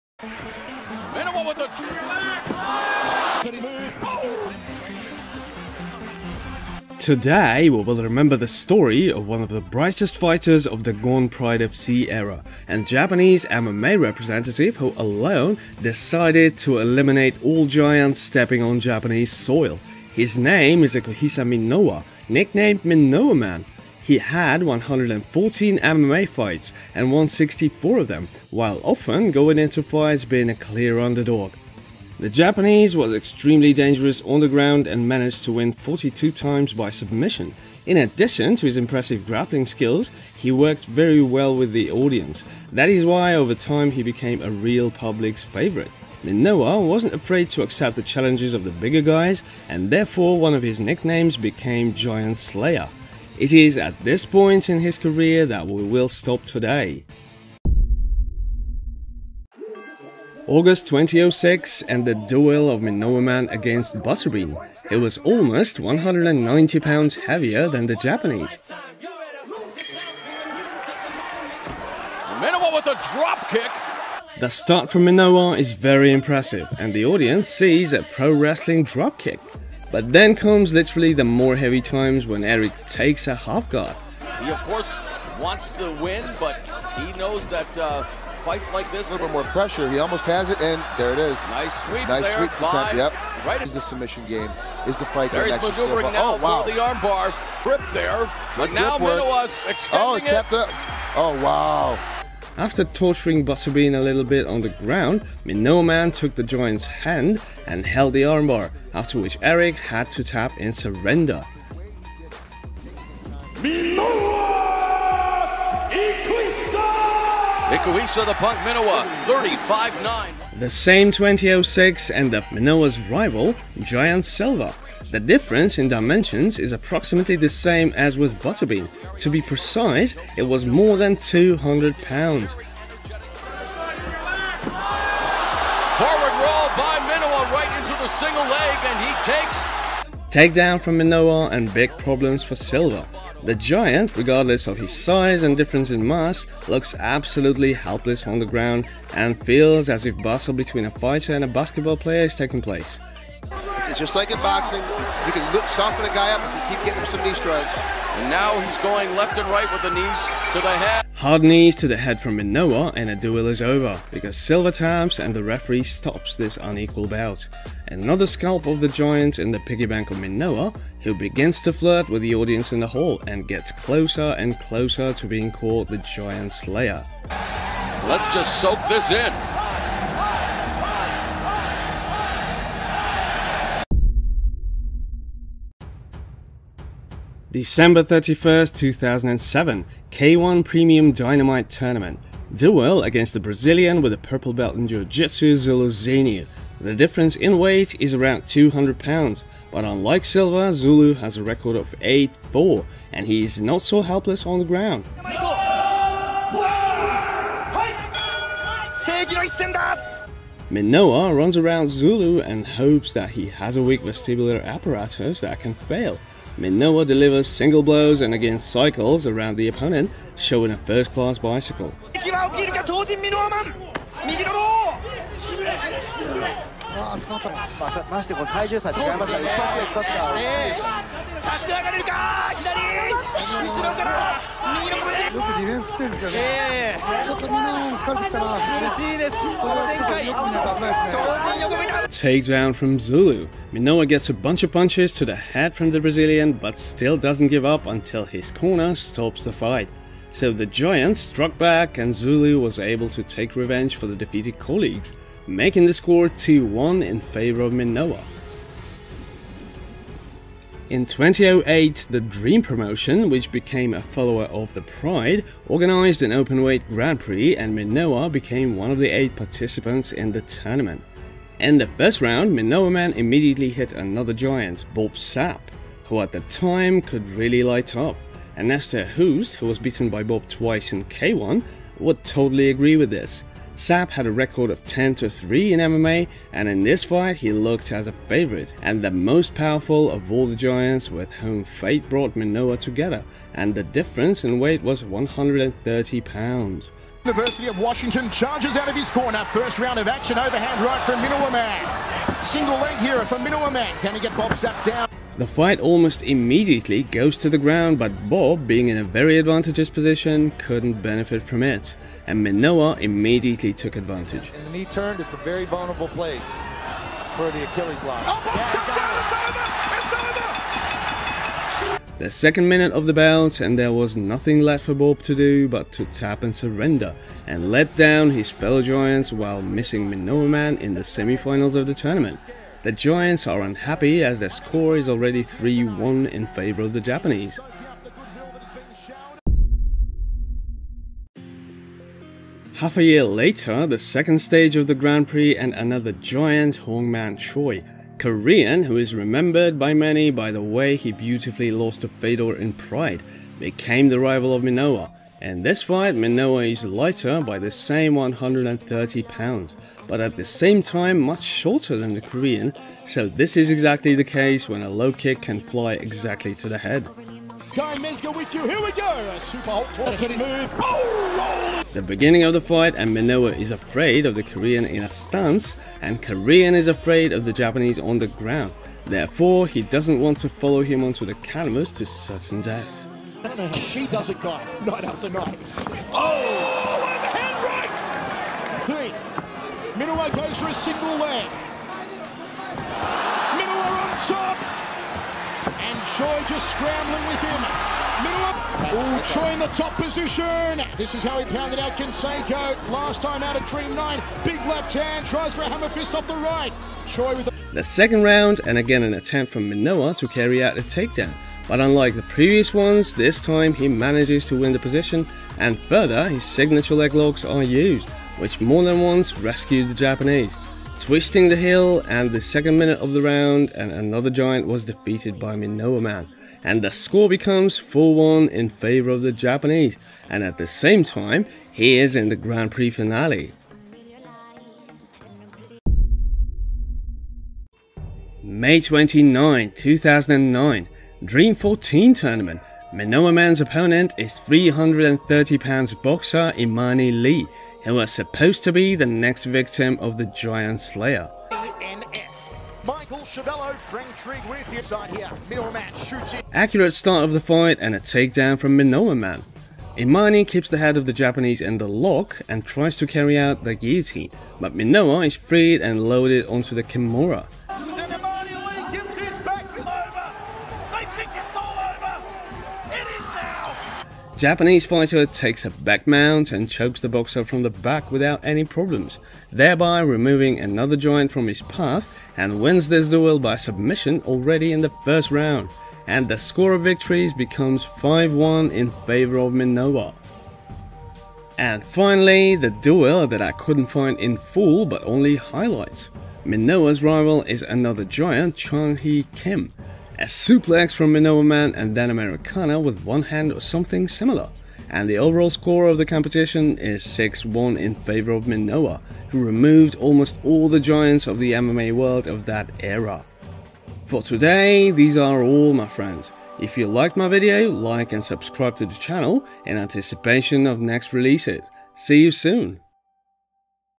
"The real GIANT KILLER" Ikuhisa Minowa | The best fights
Today we will remember the story of one the brightest fighters of the gone Pride FC era, and Japanese MMA representative, who alone decided to eliminate all giants stepping on `Japanese soil.
His name is Ikuhiso Minowa, nicknamed Minowaman. He had 114 MMA fights and won 64 of them, while often going into fights being a “clear underdog”.
the Japanese was extremely dangerous on the ground and managed to win 42 times by submission. In addition to his impressive grappling skills, he worked very well with the audience, that is why over time he became a real public’s favourite.
  Minowa was not afraid to accept the challenges of the bigger guys and therefore one of his nicknames became “giant slayer”. "It is at this point in his career that we will stop today